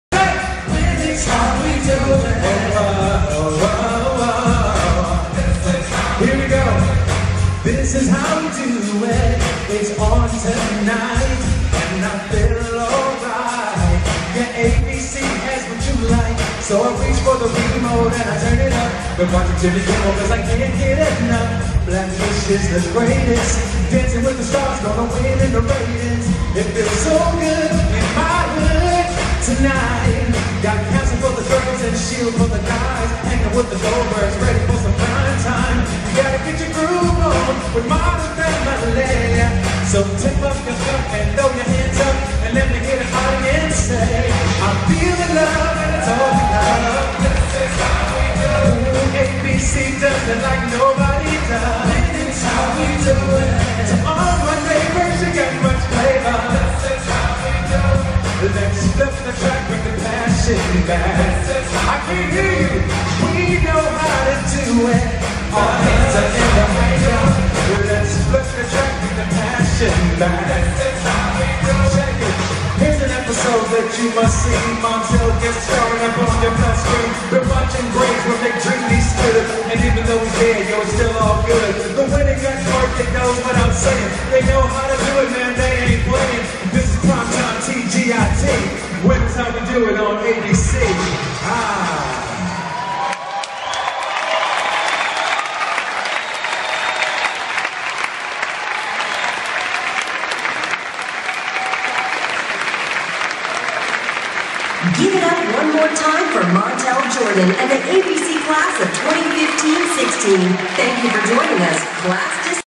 Kerry Washington & Tony Goldwyn / Dance ❤️ / ABC Upfront 2015
Kerry Washington & Tony Goldwyn ...These Two are sooo sweet! :-)))))
& Ellen Pompeo, Priyanka Chopra, Katie Lowes